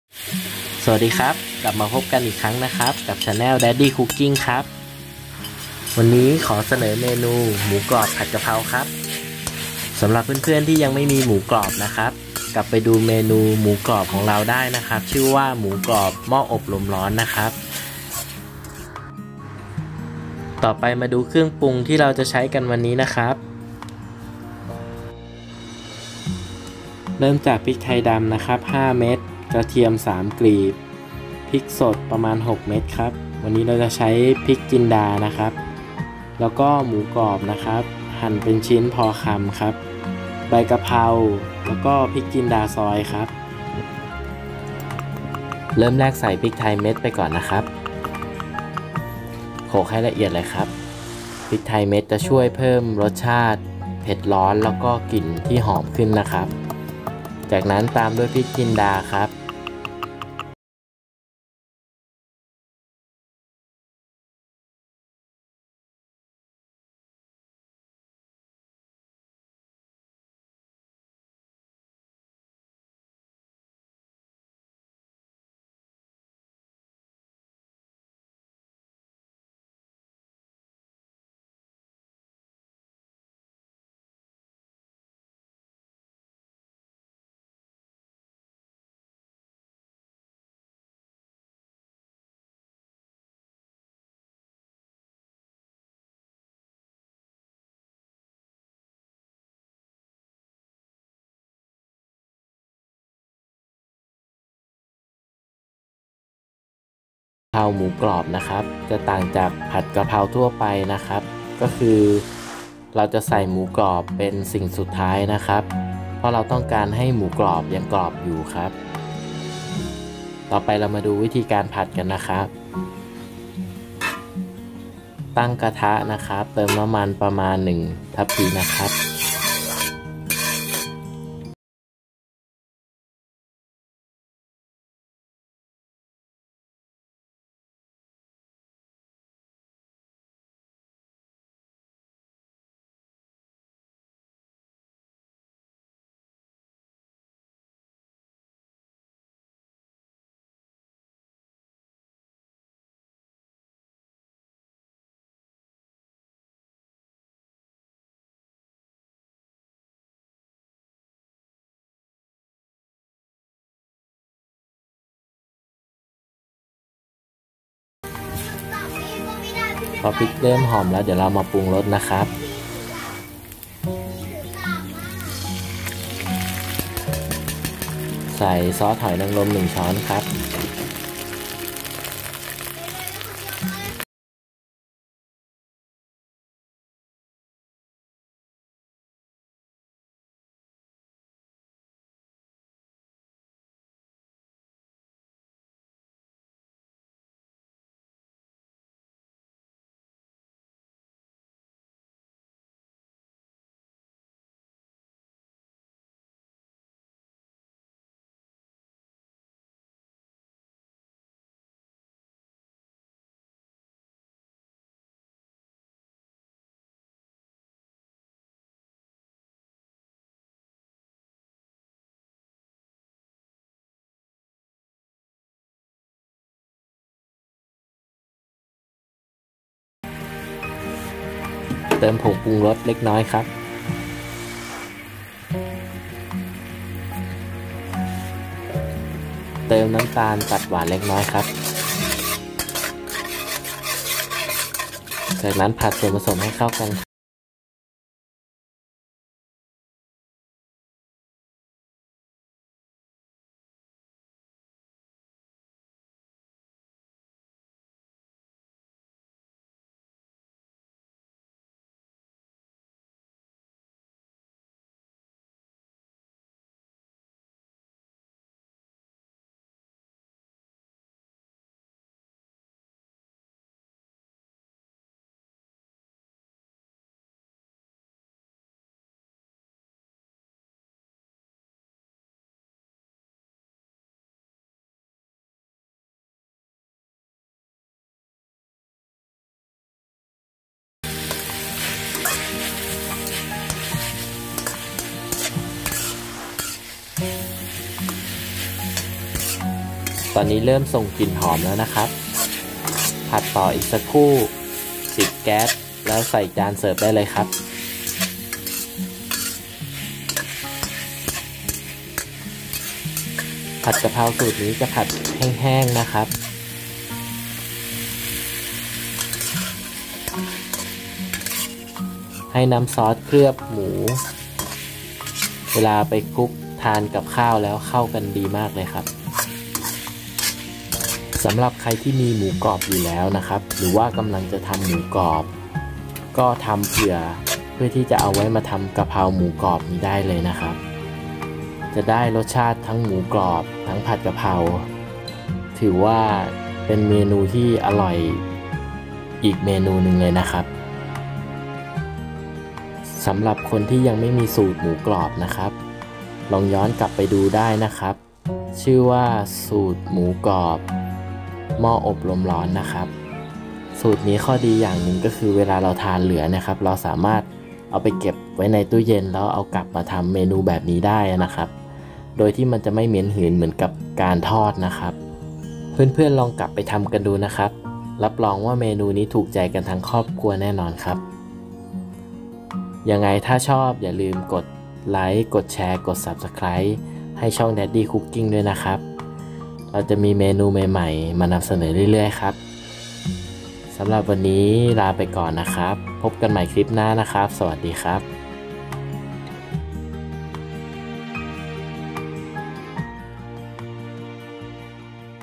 กะเพราหมูกรอบ เผ็ดๆ แห้งๆ รักหมูกรอบต้องไม่พลาด | วิธีทำ | แจกสูตร | Crispy Pork with Holy basil
ฝากติดตามช่อง daddy cooking ด้วยนะครับ
รับรองจะมีเมนูที่หลากหลายมาให้ลองทำ